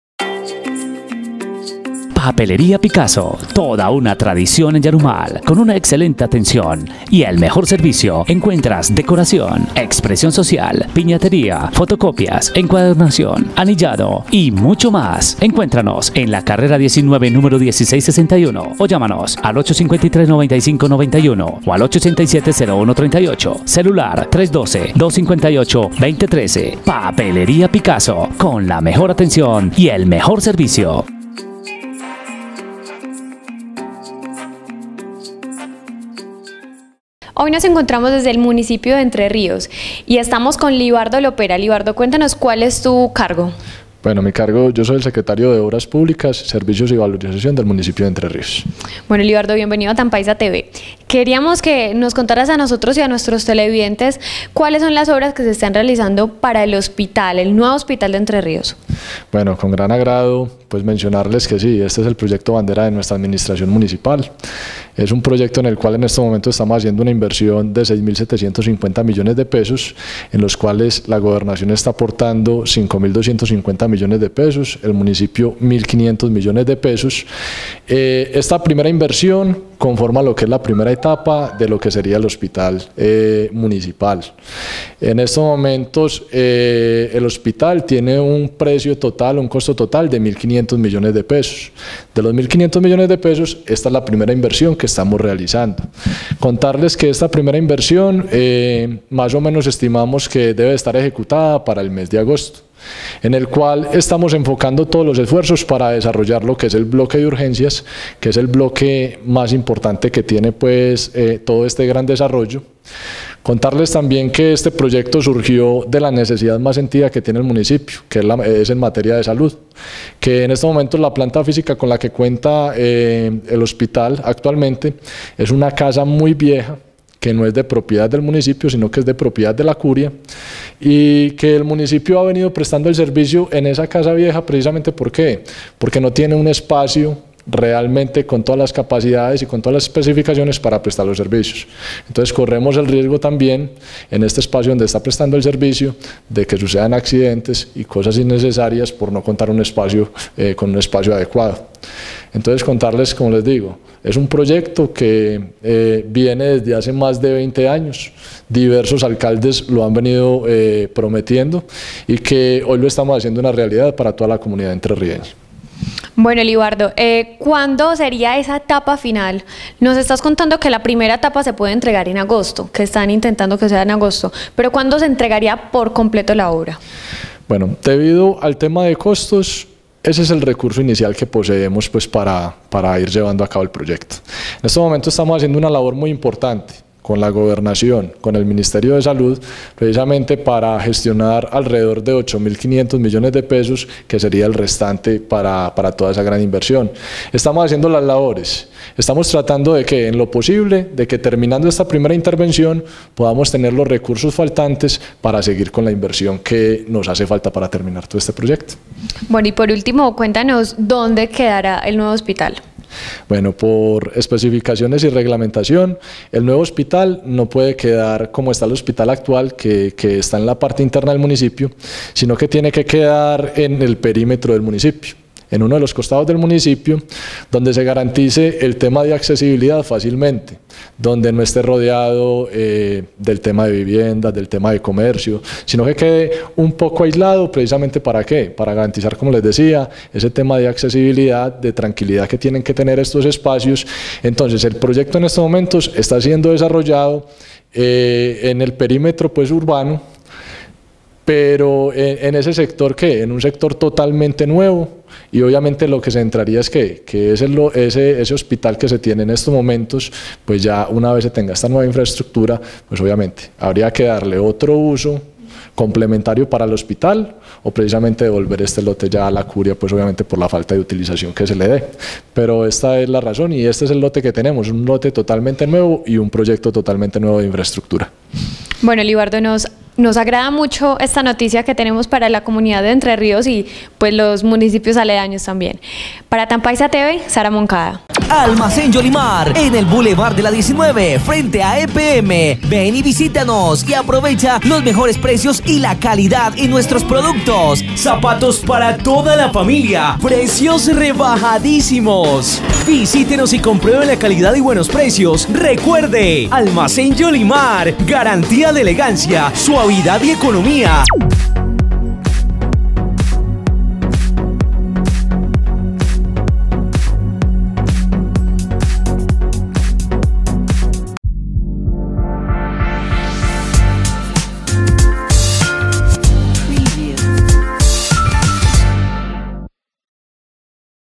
El municipio de Entrerríos tendrá un nuevo Hospital.
El municipio de Entrerríos, bajo la administración del alcalde Jorge Tamayo Villa, avanzan con su proyecto bandera, la reestructuración del hospital Presbítero Emigdio Palacio. Este proyecto está siendo financiado por la Gobernación y la administración local, los cuales buscan mejorar la atención en salud y adecuar un espacio con todas las áreas básicas necesarias en un centro de salud. Se planea entregar la primera fase del proyecto en el mes de agosto.